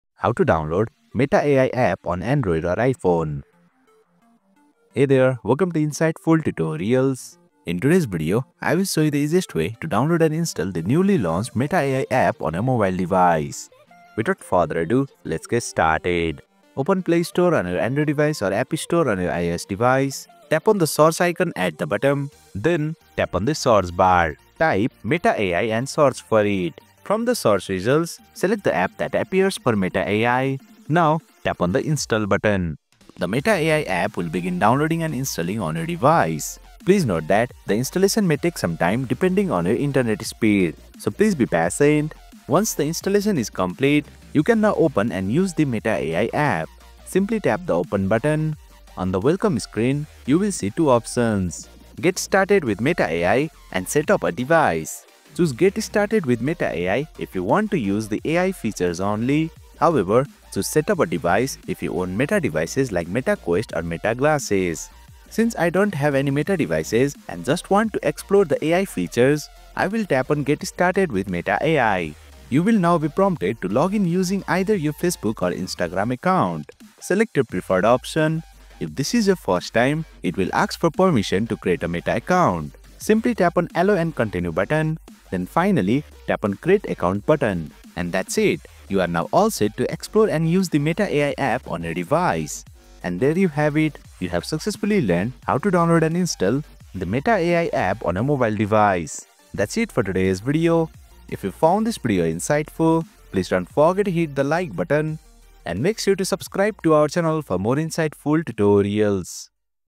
How To Download Meta AI App on Android/iPhone (2025) | Install Meta AI App
Want to try Meta AI on your phone? In this quick guide, I’ll show you how to download and install the Meta AI app on both Android and iPhone devices in 2025. Whether it's from the Play Store or App Store, this tutorial covers it all step by step.

🔹 Tips:
✅ Make sure you're using the latest version of the Play Store or App Store.
✅ Use the official app from Meta to avoid scams or fakes.

⏳ Timestamps:
00:00 - Intro
00:20 - Open Play Store / App Store
00:40 - Search “Meta AI” and find the official app
01:05 - Tap Install and wait for download
01:20 - Open and set up Meta AI

Stay updated with the latest app guides, feature updates, and helpful tips for Android and iPhone!